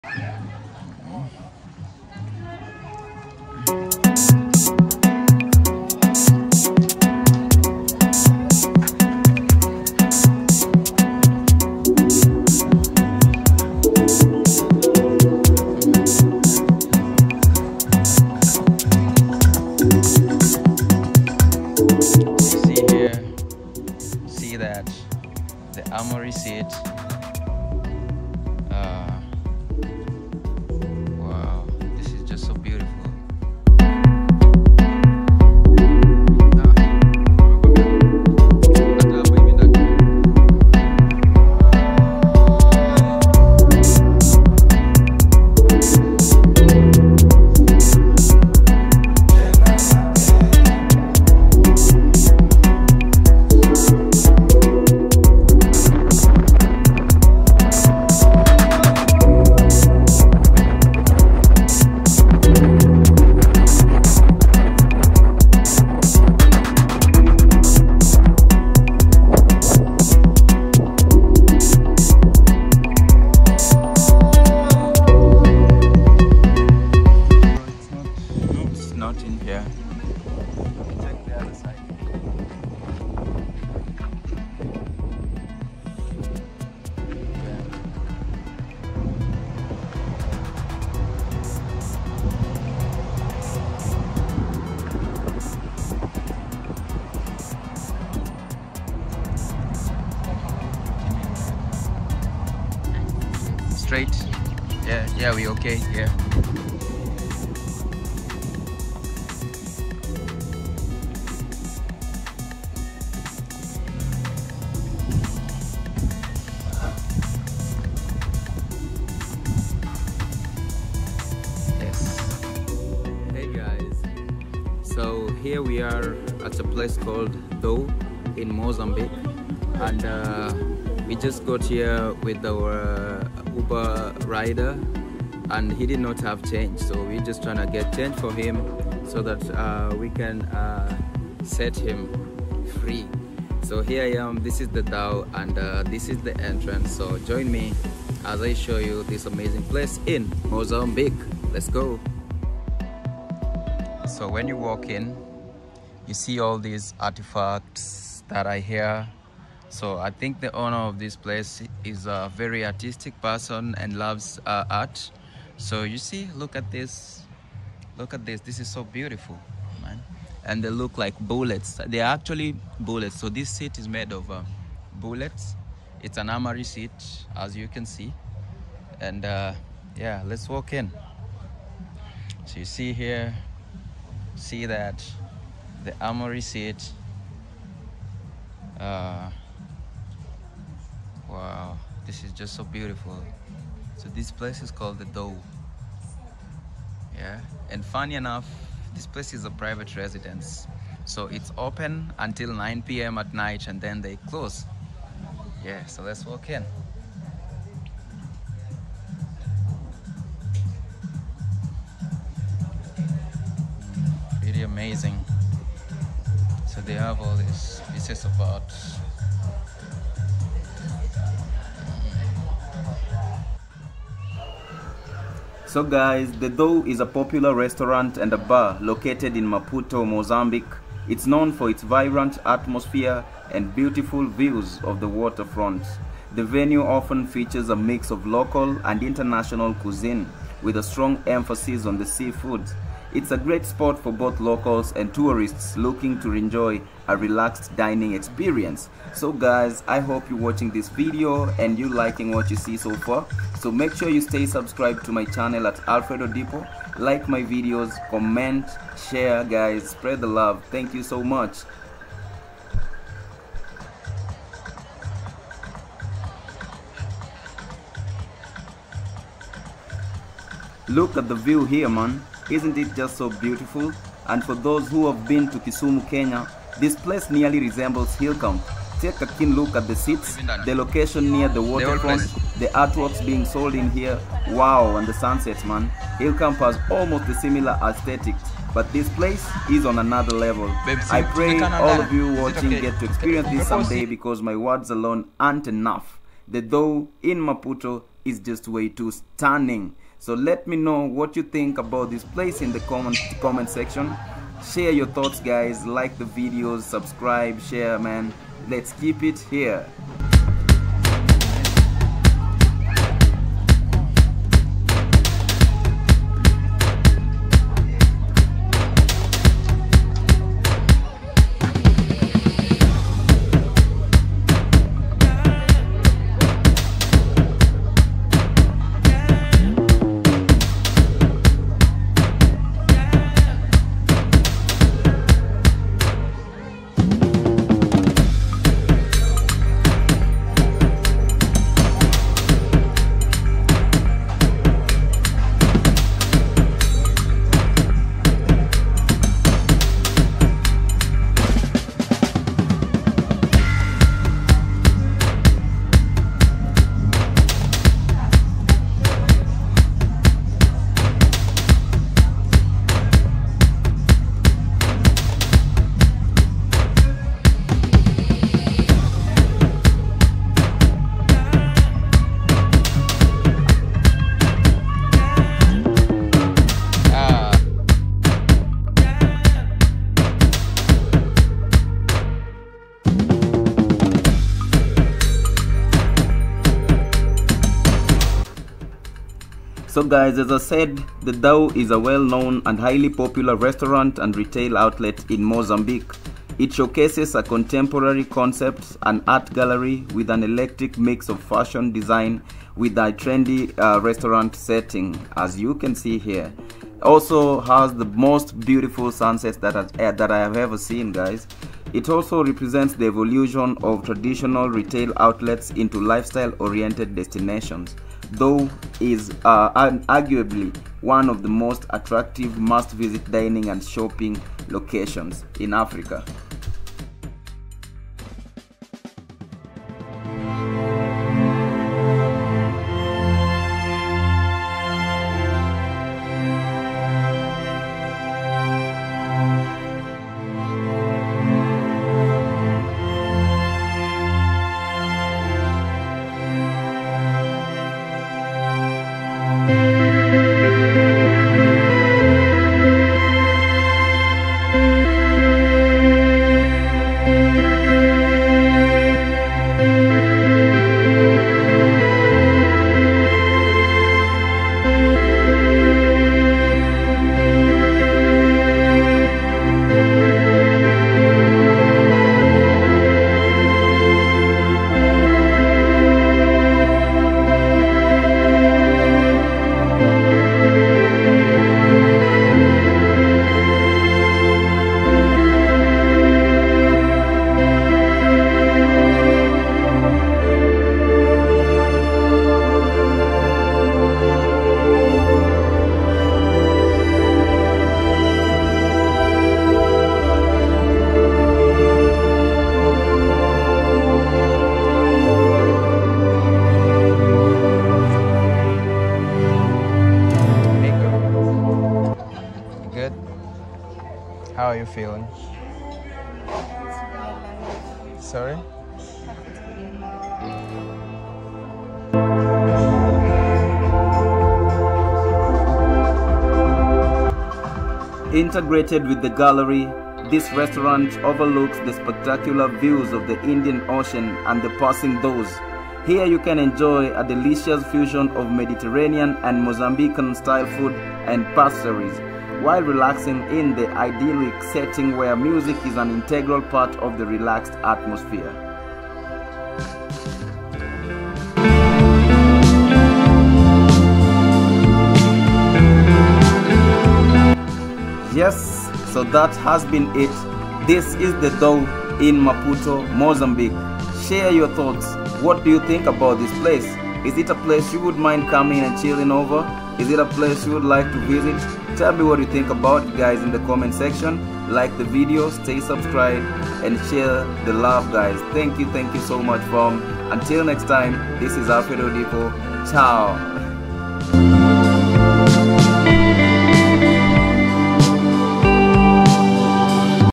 Unveiling the Dhow Maputo || A voyage of culture and beauty
DHOW is a highly popular restaurant and retail outlet in Mozambique, representing the evolution of traditional retail outlets into lifestyle oriented destinations. It is one of Africa's most attractive 'must visit' dining and shopping locations.